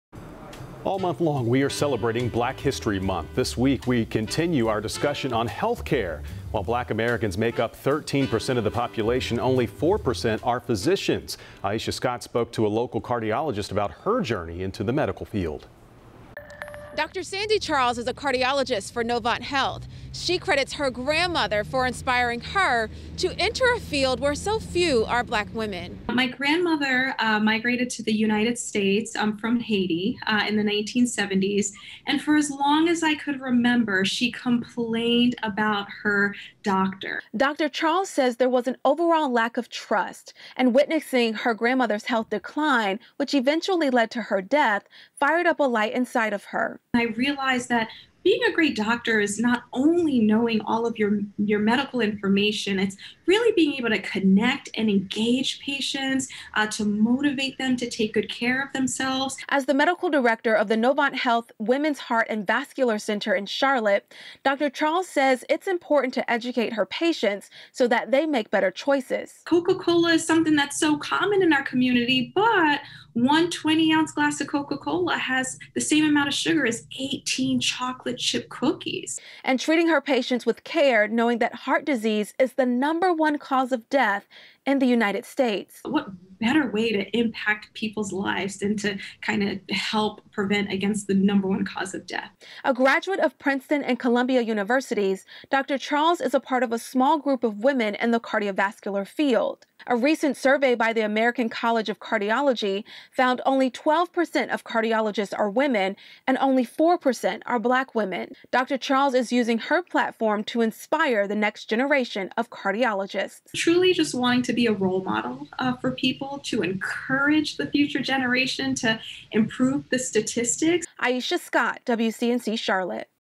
Highlighting Doctor Sandy Charles, a cardiologist for Novant Health
Only 12% of cardiologists are women and only 4% are Black women.